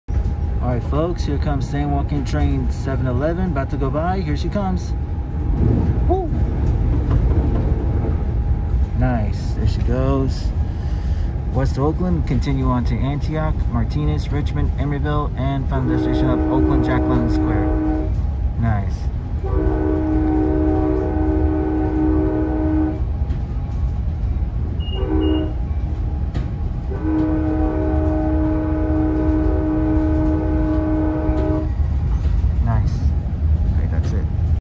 Amtrak San Joaquin Train 711 Northbound To OKJ Passing By At Knightsen In 4K
Leading The Train is Siemens SC44 CDTX 2102 & Cabcar CDTX 8308 In The Rear